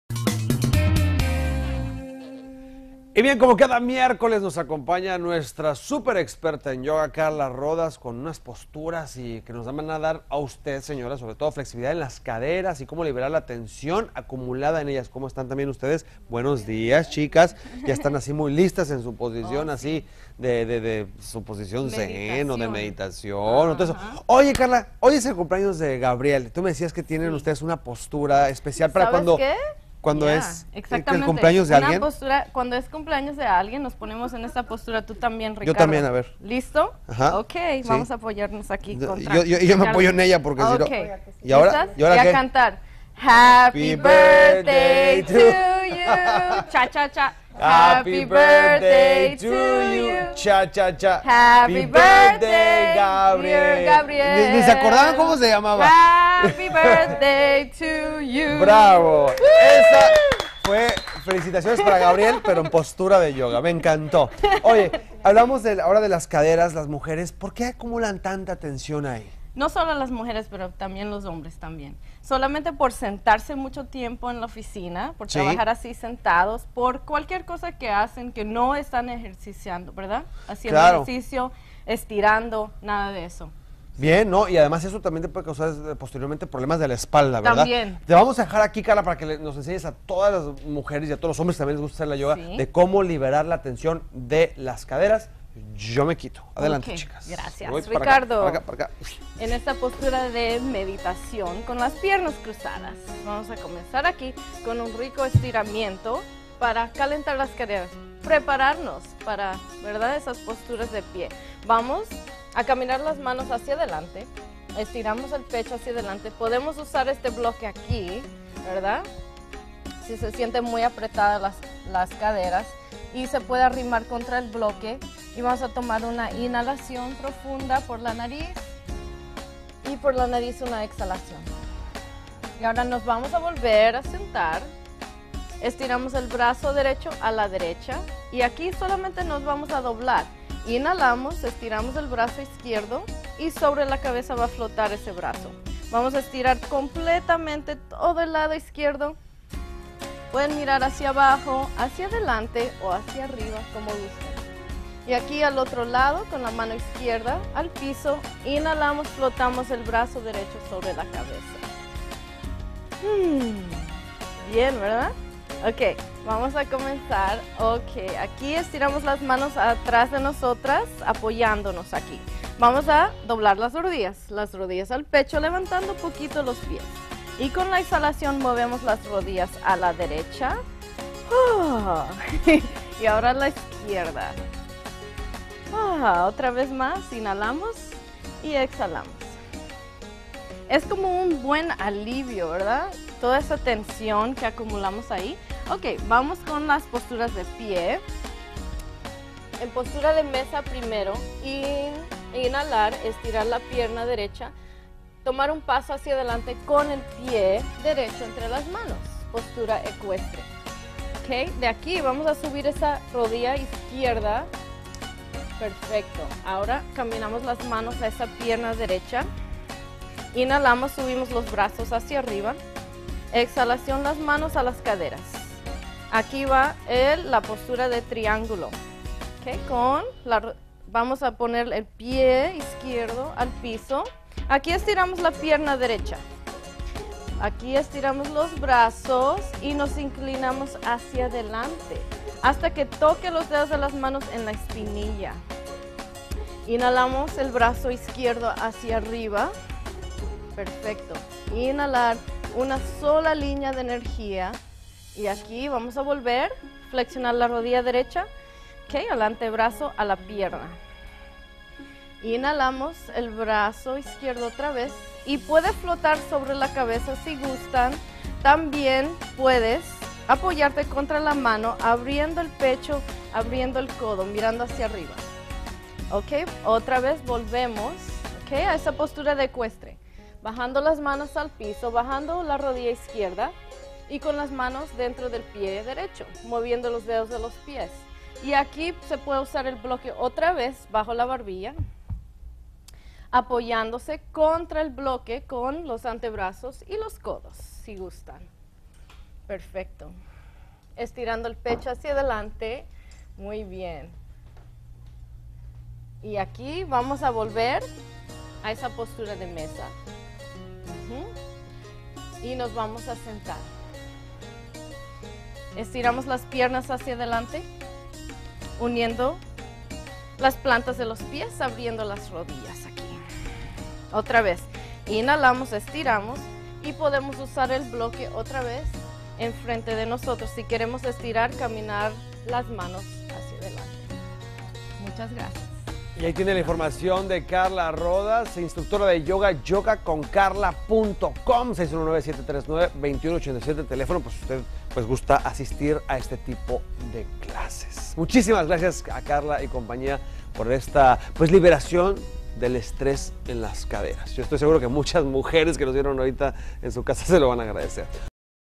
YOGA CON KARLA RODAS PARA LIBERAR TENSION
COMO CADA MIERCOLES KARLA NOS MUESTRA COMO LIBERAR TENSION EN LAS CADERAS Y DESESTRESARNOS